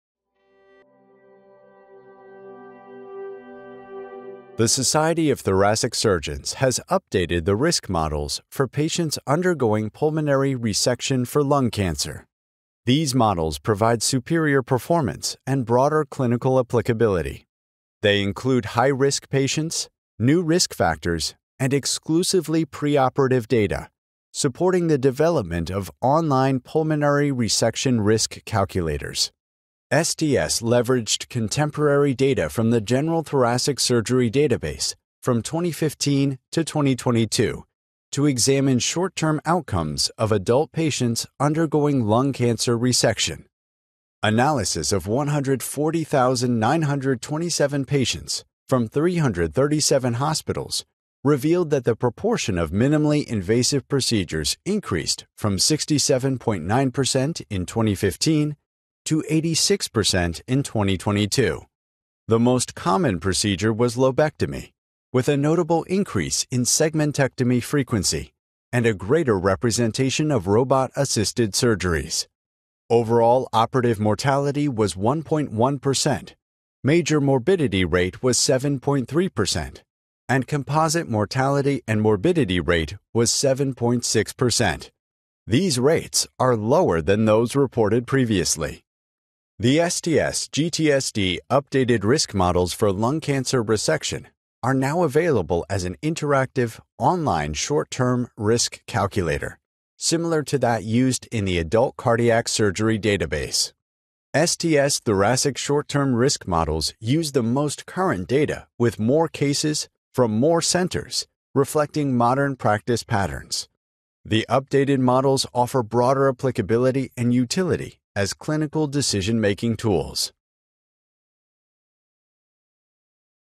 Lung Resection Risk Calculator
The new pulmonary resection for lung cancer Risk Calculator is engineered using contemporary data from the STS General Thoracic Surgery Database. It allows surgeons and multidisciplinary medical providers to estimate a patient’s individualized risk in real-time, based on specific factors such as tumor characteristics, comorbidities, and procedures performed. As patient factors are added by the user, the risk estimates of morbidity and mortality are dynamically calculated along with 95% confidence intervals. Try the new calculator: